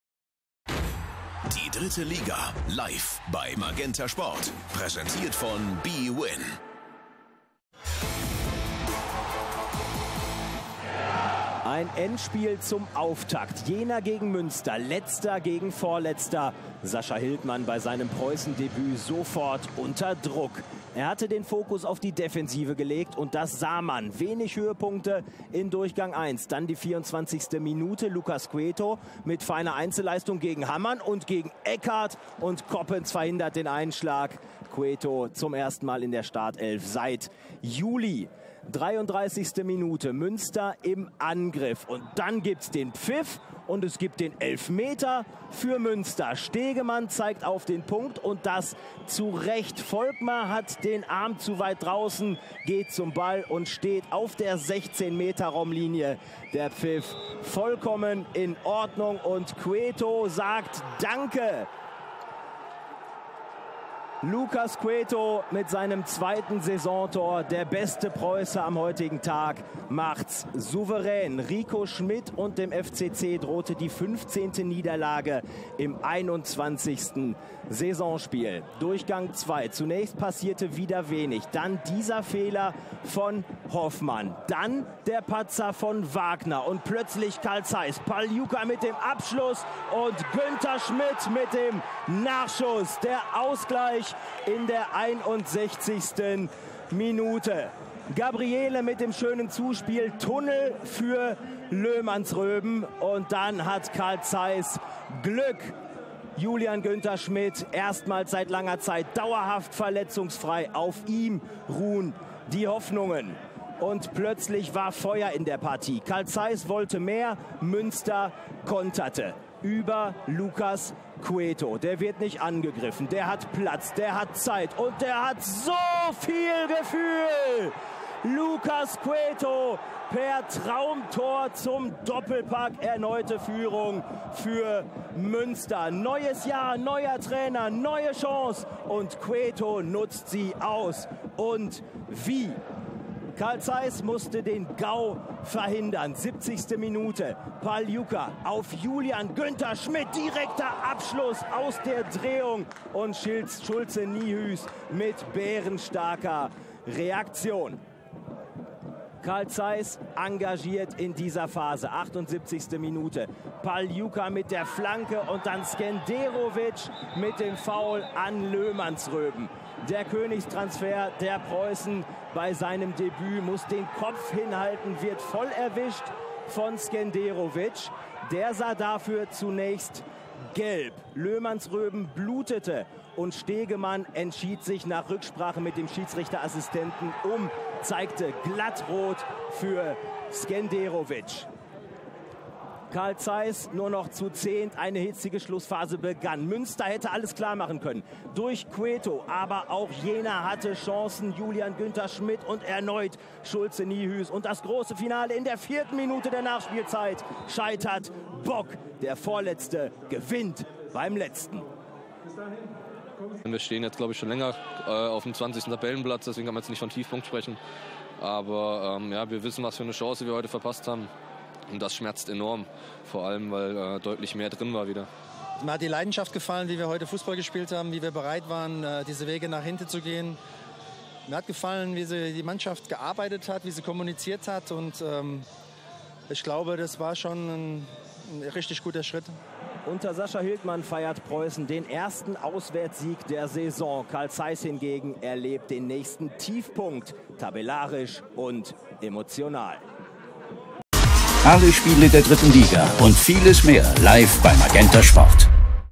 FC Carl Zeiss Jena - Preußen Münster | Spieltag 21, 19/20 | MAGENTA SPORT
Alle Spiele der 3. Liga live bei MagentaSport! Jetzt buchen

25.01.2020, 21. Spieltag, 19/20. Münster gewinnt im Kellerduell.
Im direkten Aufeinandertreffen des Tabellenletzten und dem Vorletzten können sich die Münsteraner bei Lucas Cueto bedanken, der mit seinem Doppelpack das Spiel zu Gunsten der Gäste entschied (33., 68.).
Engagierte Jenaer glichen zwar durch Julian Günther-Schmidt aus, für den wichtigen Punktgewinn reichte es jedoch nicht.

FC Carl Zeiss Jena:
Coppens, Grösch, Volkmer (Bock), Obermair, Eckardt (Skenderovic), Stanese, Fassnacht, Gabriele (Käuper), Günther-Schmidt, Pagliuca

Preußen Münster:
Schulze Niehues, Scherder, Löhmannsröben, Erdogan, Schauerte, Wagner, Rodrigues Pires (Brandenburger), Hoffmann, Cueto, Schnellbacher (Königs), Litka (Özcan)

Bleib' immer am Ball und verpasse keine Highlights mehr: MagentaSport MagentaSport auf YouTube MagentaSport auf Facebook MagentaSport auf Instagram